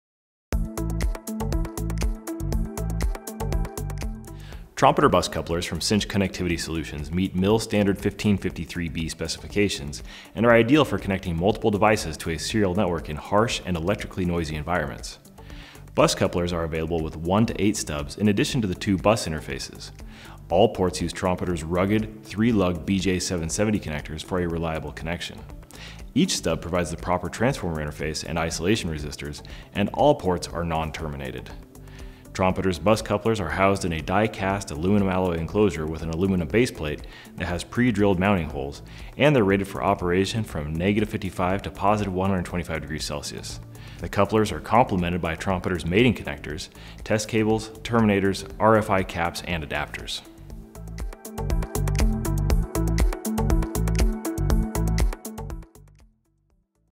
Trompeter/Cinch Connectivity Solutions MIL-STD-1553B Bus Couplers — New Product Brief | Mouser
Trompeter / Cinch Connectivity Solutions TEBC Bus Couplers meet MIL-STD-1553B specifications and come in configurations from one stub to eight stubs.